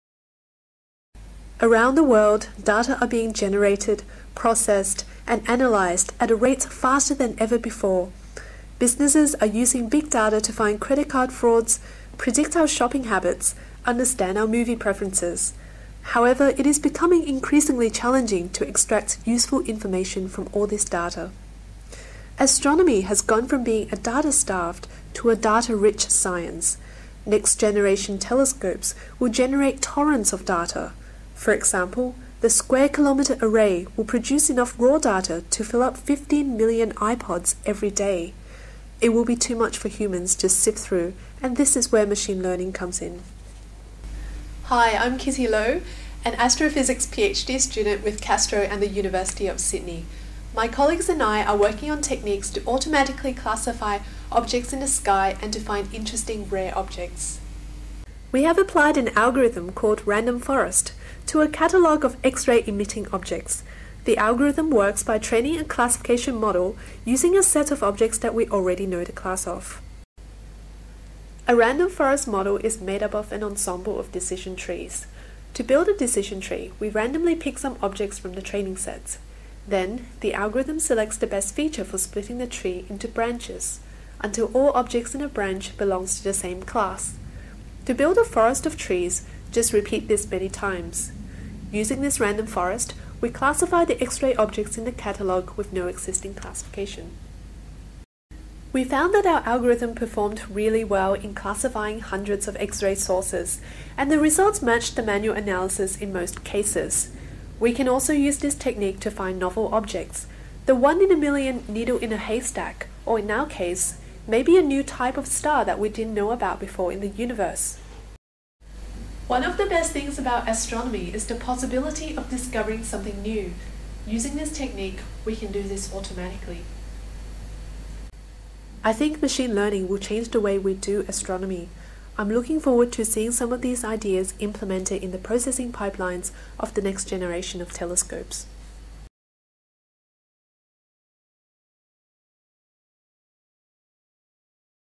Computers beat brainpower when it comes to counting stars
A team of University of Sydney astronomers has developed a new way to automatically classify huge numbers of astronomical objects, and to discover new, exotic ones almost as soon as they happen.
Massive torrents of raw data are now collected by telescopes on a daily basis creating an urgent need to massively accelerate the reliable classification of millions of stars and galaxies, and to quickly highlight objects that might be new discoveries or that have unusual properties.

Kitty K. Lo, Sean Farrell, Tara Murphy, B. M. Gaensler "Automatic classification of time-variable X-ray sources", publication in the Astrophysical Journal

Image and video credits, in order of appearance:

Jimdo.de
Thierry Gregorius
Edo Bakker / POstNL
Kenneth Freeman
The Opte Project
DARPA
Hevelius
HJ Detouche
ESO / José Francisco Salgado
Dragonfly Media
NASA
Swinburne Astronomy Productions
Alain Fleischer
Nando de Freitas
NASA
NASA-CXC
NASA
Swinburne Astronomy Productions
W Rebel
IMeowbot

Video produced by Dr Wiebke Ebeling, CAASTRO Education & Outreach